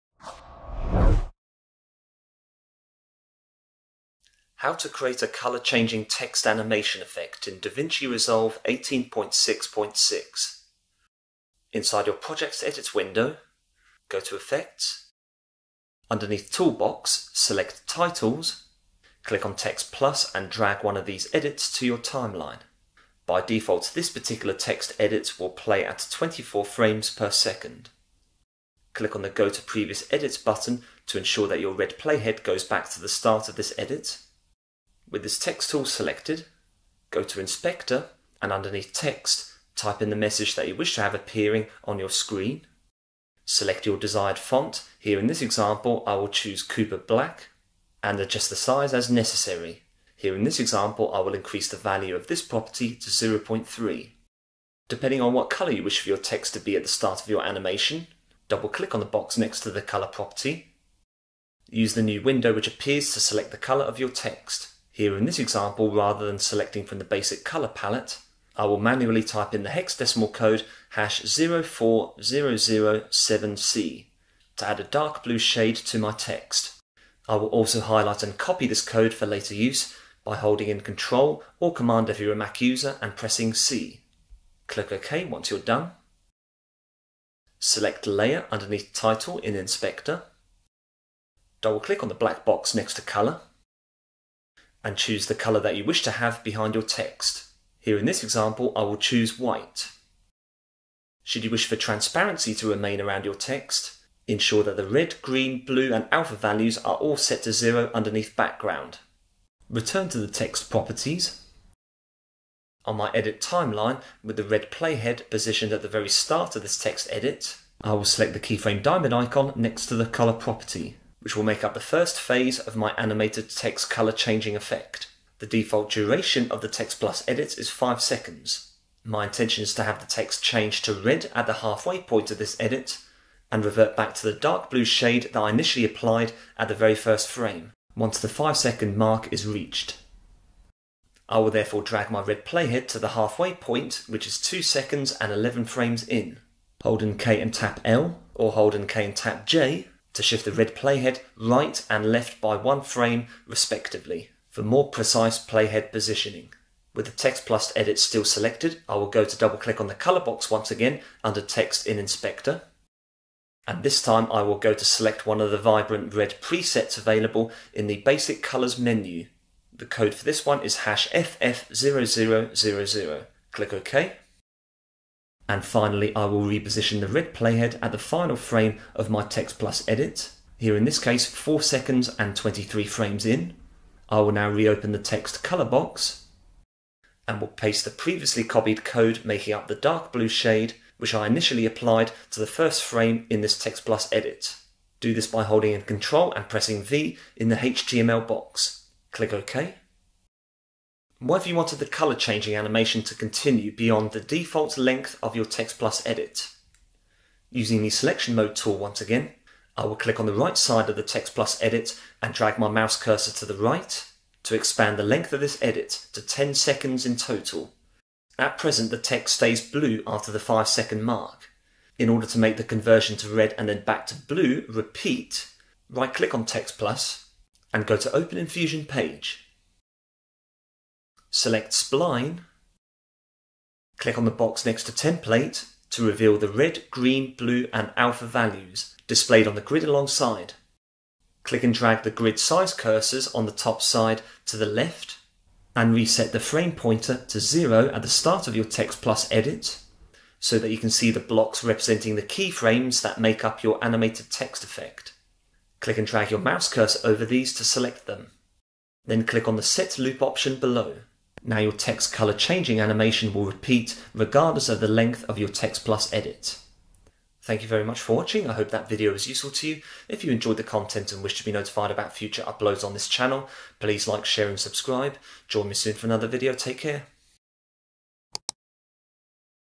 DaVinci Resolve: How To Create Animated Colour Changing Text
DaVinci Resolve Tutorial #229. In this video, you will learn how to make the colour of text blend to a different shade. Version 18.6.6 of the software is used in this tutorial.

***SPECIAL OFFERS

3) VISUALVFX

C0NTI